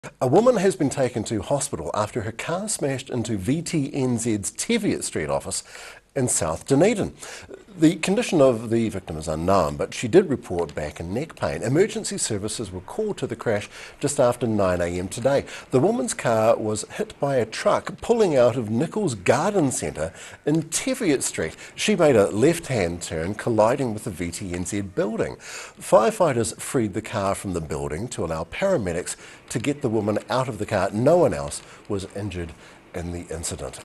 Car slams into VTNZ office
A woman has been taken to hospital after her car smashed into VTNZ's Teviot St office in South Dunedin.

The condition of the victim is unknown, but she did report back and neck pain.

Emergency services were called to the crash just after 9am today.